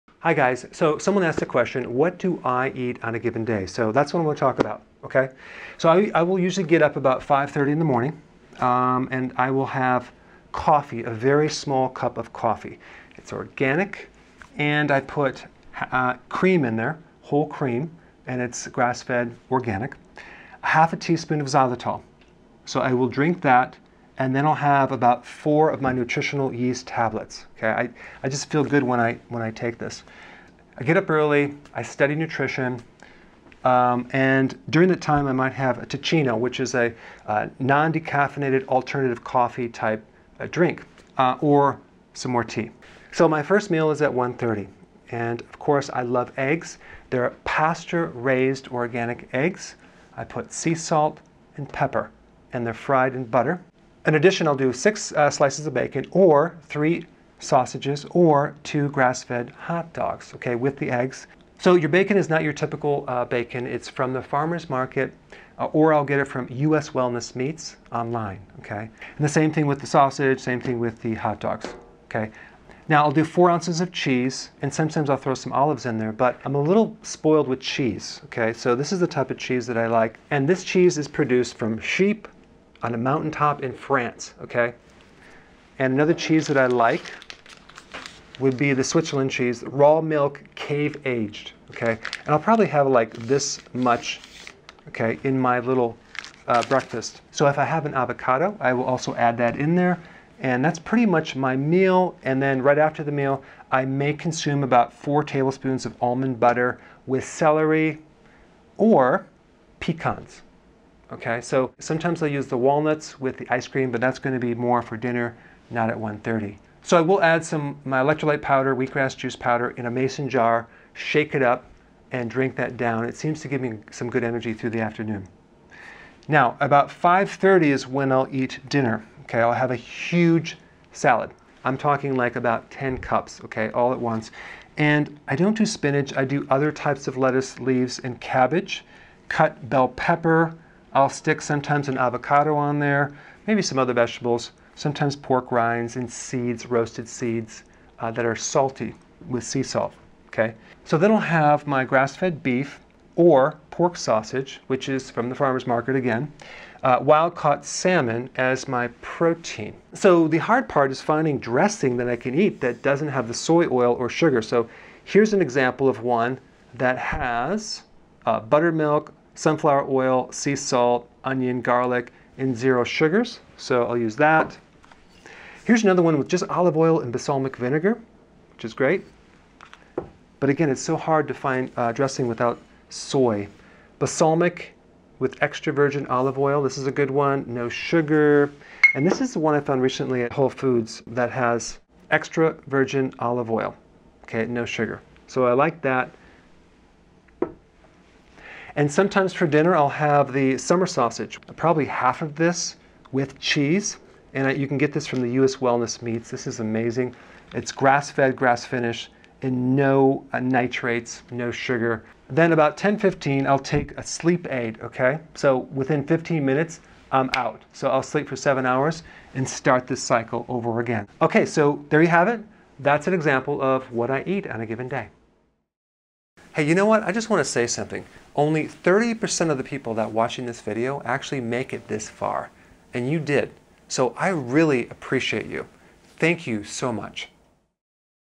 What Keto Foods Does Dr. Berg Eat? – Dr. Berg
Wondering what I eat in a day? Watch this video for some keto meal ideas.

0:00 Introduction: What Dr. Berg eats in a day
0:45 Dr. Berg meal ideas
2:05 What Dr. Berg eats for dinner
3:45 Thanks for watching!

In this video, I’m going to go over everything I eat in a day on a keto diet. I usually wake up around 5:30 a.m. and have a small cup of coffee with cream and a ½ teaspoon of xylitol. I’ll also have 4 nutritional yeast tablets and either Teeccino or tea later on.

I have my first meal around 1:30 p.m. Typically I’ll have 4 pasture-raised eggs seasoned with sea salt and pepper. With my eggs, I’ll have either 6 slices of bacon, 3 sausages, or 2 grass-fed hot dogs. I’ll also eat 4 ounces of my favorite cheese with some olives. Sometimes I’ll add in an avocado as well.

Right after the meal, I’ll consume about 4 tablespoons of almond butter with celery or some pecans. I usually mix electrolyte powder and wheatgrass juice powder and drink it for energy.

Around 530 p.m. I’ll have dinner. I always have a huge salad of around 10 cups of vegetables. I don’t use spinach. I’ll incorporate other types of lettuce as well as cabbage. I like to add bell pepper, avocado, pork rinds, and seeds. Always choose a salad dressing without sugar! Alongside my salad, I’ll have some protein. I have either grass-fed beef, pork sausage, or wild-caught salmon.

At 10:15 I’ll take Sleep Aid, which helps me fall asleep within 15 minutes. I’ll sleep for 7 hours and do it all over again!

I hope this gives you some ideas about what to eat on keto.